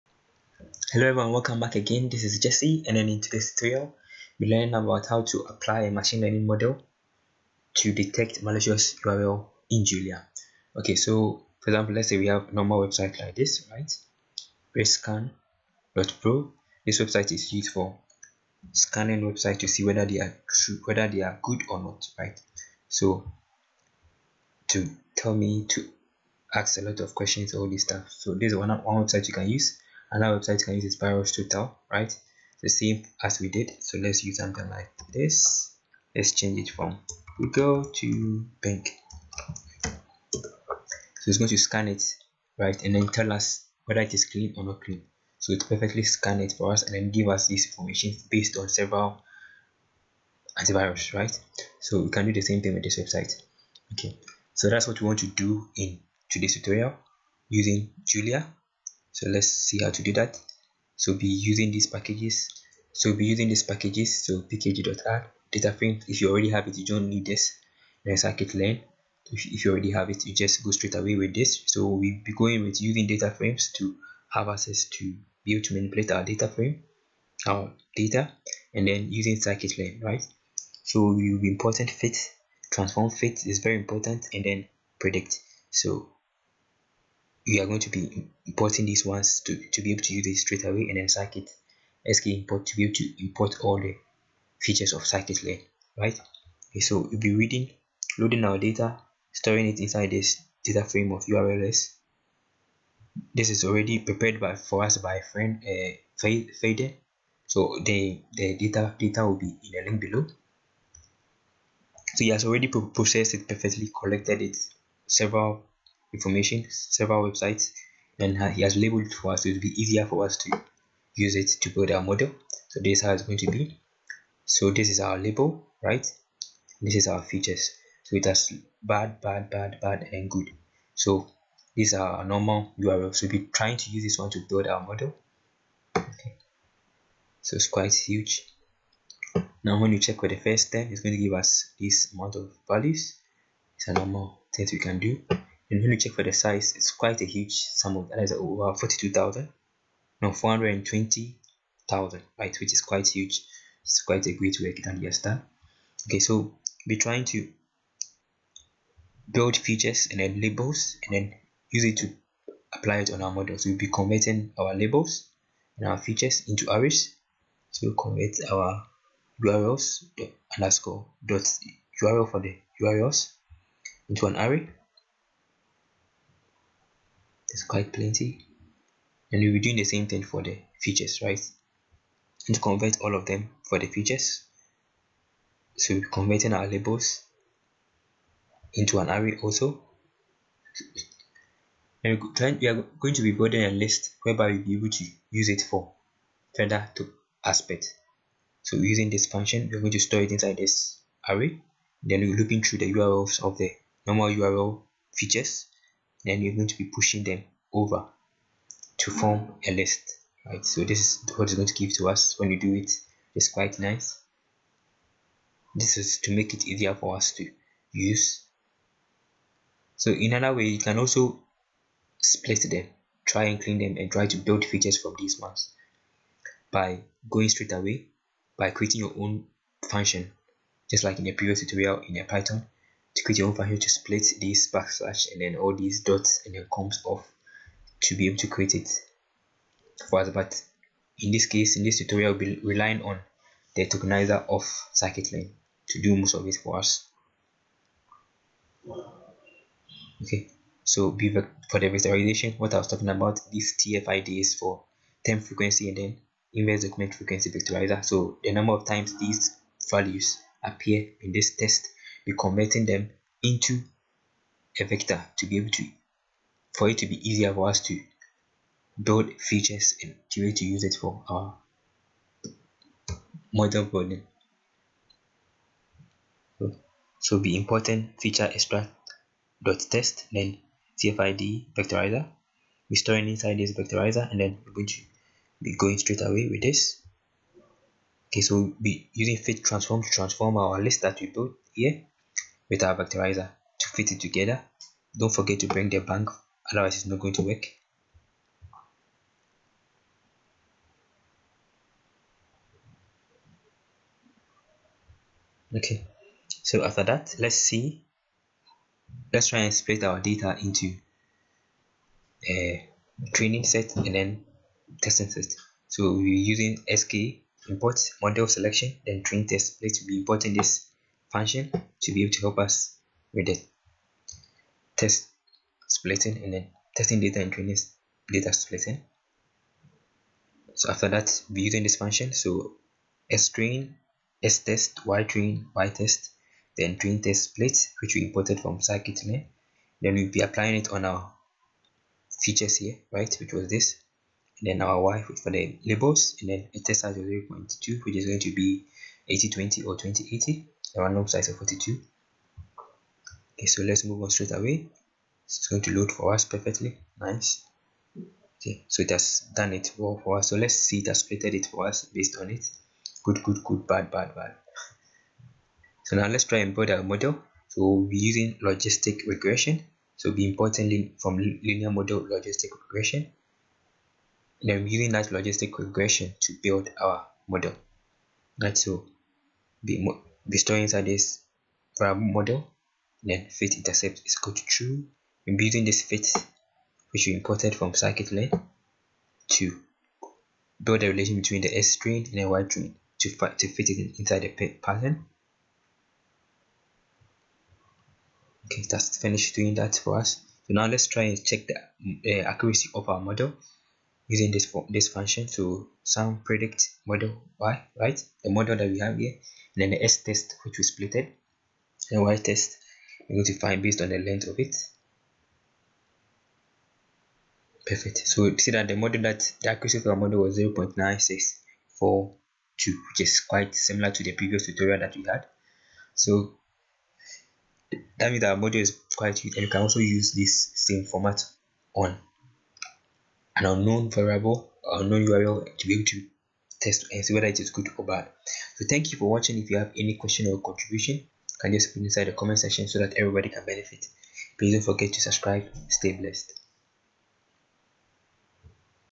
Detecting Malicious Urls with Machine Learning in Julia
In this tutorial we will be discussing how to detect malicious urls or websites using machine learning in Julia.
We will build a model and use it on our dataset